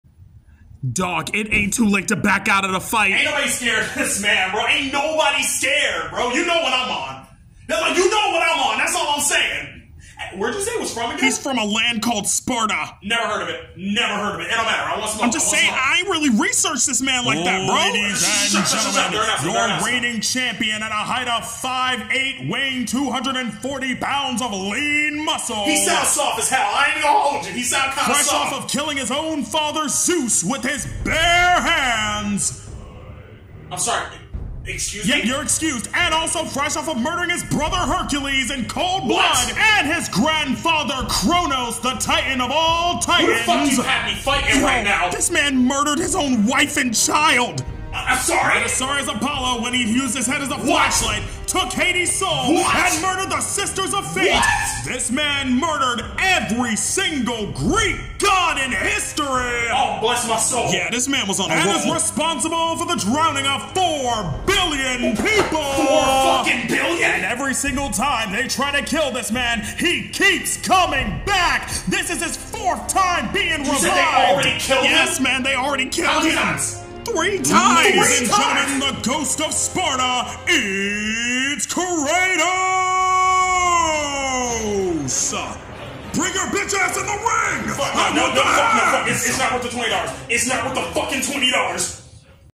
When You Don't Research Your Opponent (Tribute!)
proooobably should've looked up who he was fighting!

this is based on the series created by the amazing tiktoker Aeoxis!